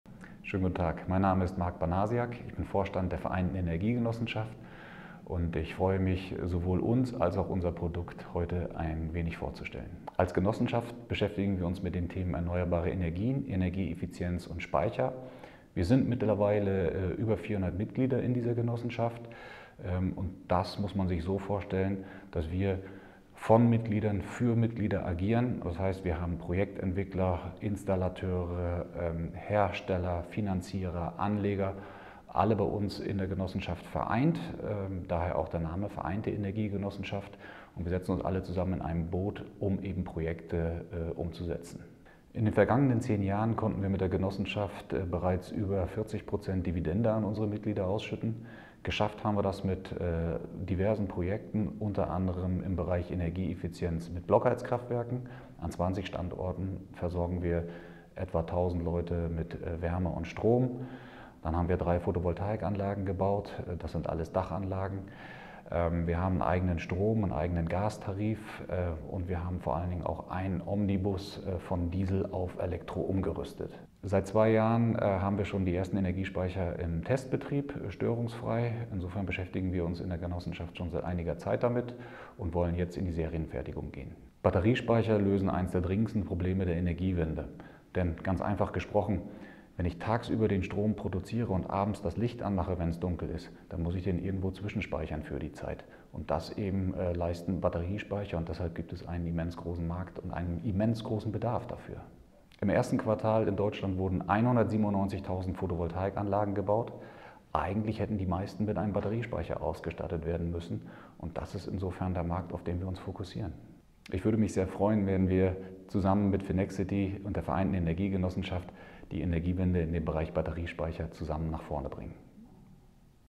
VEG EnergyHub | Deal Talk mit Marc Banasiak, Vorstand der VEG eG | Renewables Investment
Die Vereinte Energiegenossenschaft eG ist seit Ihrer Gründung im Jahr 2013 stetig gewachsen. Mit über 400 Mitgliedern hat sie im Bereich der dezentralen Energieversorgung ein Portfolio aus 20 hocheffizienten Blockheizkraftwerken, neun Kesselprojekten sowie drei Photovoltaikanlagen aufgebaut. Mit ihrem ersten Projekt auf FINEXITY strebt die VEG die Finanzierung einer Serienfertigung für Energiespeicher an.

Kapitelauswahl:
00:00 - Vorstellung der VEG durch Vorstand Marc Banasiak
0:42 - Trackrecord der VEG
1:15 - Energiespeicher und das Projekt "EnergyHub"
1:42 - Marktausblick

Anbieter: Vereinte Energiegenossenschaft eG (VEG)
Stand: 07/2023

Gesetzlicher Risikohinweis: Der Erwerb dieser Wertpapiere ist mit erheblichen Risiken verbunden und kann zum vollständigen Verlust des eingesetzten Vermögens führen.